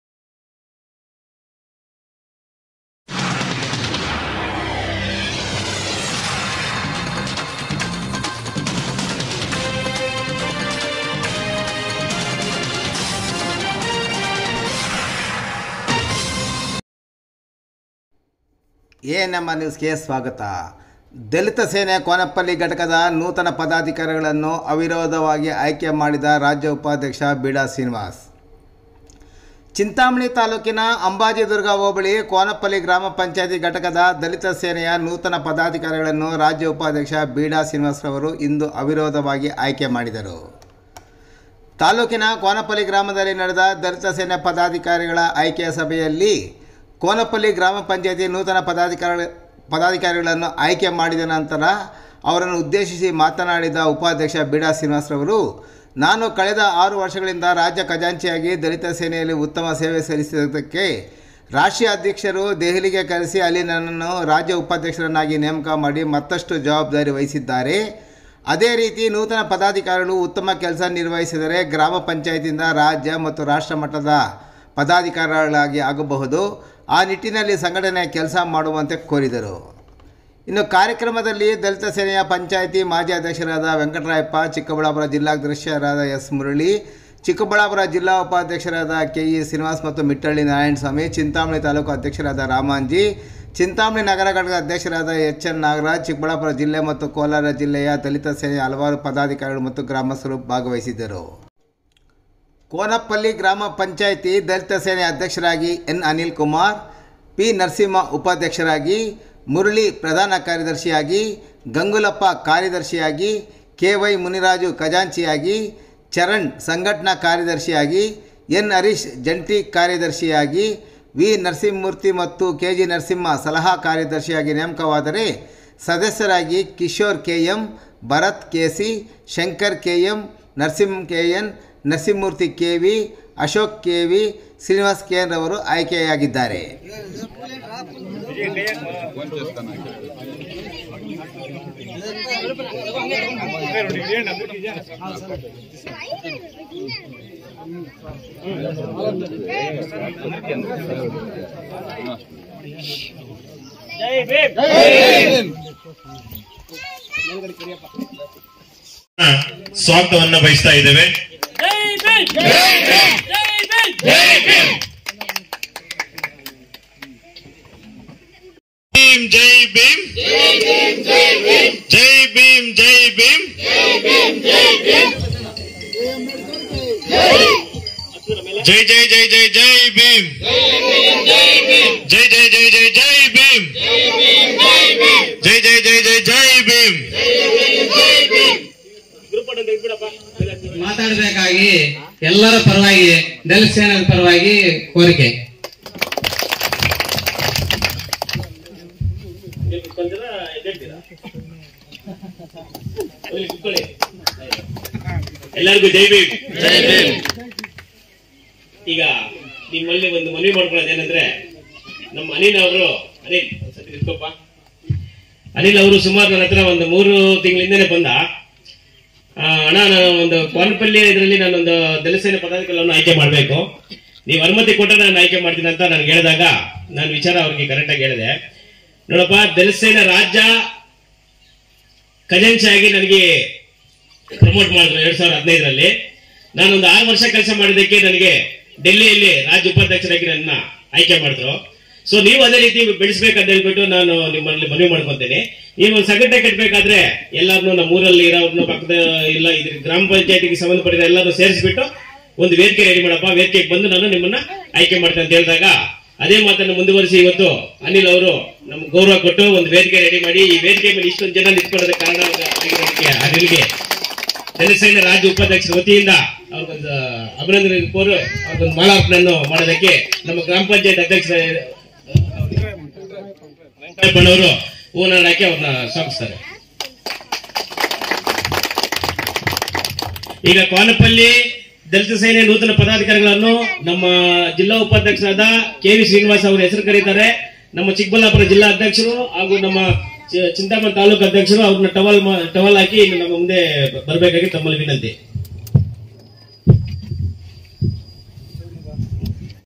ದಲಿತ ಸೇನೆ ಕೋನಪಲ್ಲಿ ಘಟಕದ ನೂತನ ಪದಾಧಿಕಾರಿ ಗಳನ್ನು ಅವಿರೋಧ ಅಯ್ಕೆ ಮಾಡಿದ ರಾಜ್ಯ ಉಪಾಧ್ಯಕ್ಷ ಬೀಡಾ ಶ್ರೀನಿವಾಸ್
ದಲಿತ ಸೇನೆ ಚಿಂತಾಮಣಿ ತಾಲ್ಲೂಕಿನ ಕೋನಪಲ್ಲಿ ಘಟಕದ ನೂತನ ಪದಾಧಿ ಕಾರಿಗಳನ್ನು ಅವಿರೋಧ ಅಯ್ಕೆ ಮಾಡಿದ ರಾಜ್ಯ ಉಪಾಧ್ಯಕ್ಷ ಬೀಡಾ ಶ್ರೀನಿವಾಸ್